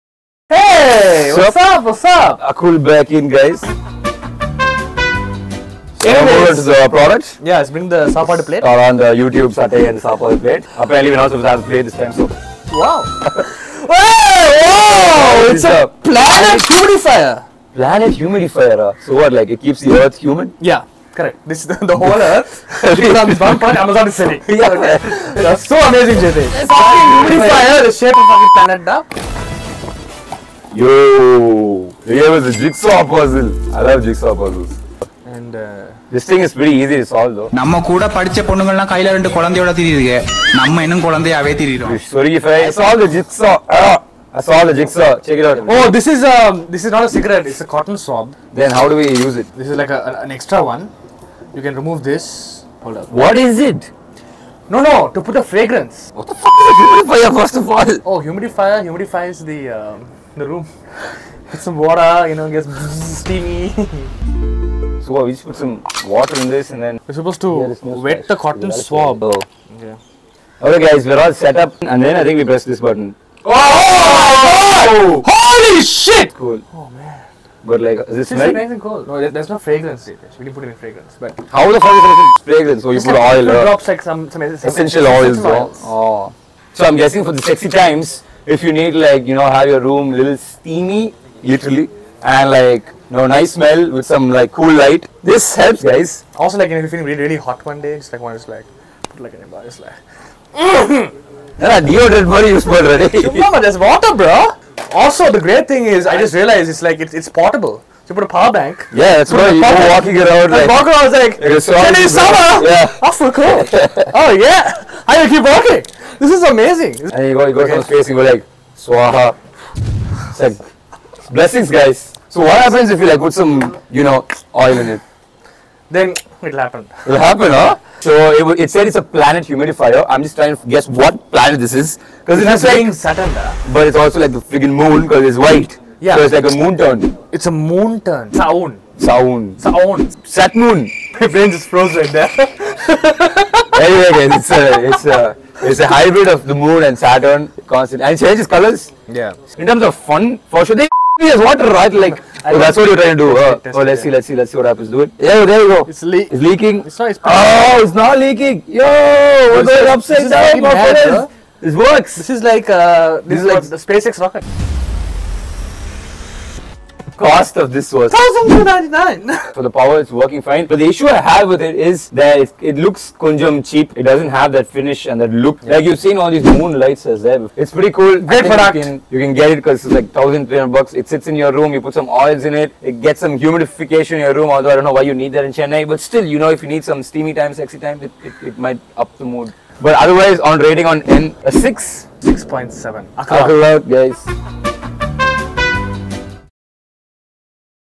Is this a Planet or a Humidifier??!! | Akkul Bros
This time on Akkul Bros, Jay & Mavi review a planet shaped humidifer, who thinks of contraptions like this? we have no idea but we intend to get to the bottom of finding out whether it is useful or not!

Hosts:
Jaytesh Calpakkam (@jayteshc)
Mavisha Ramachandran (@grizzlyislander)

Comment, share and support folks 👏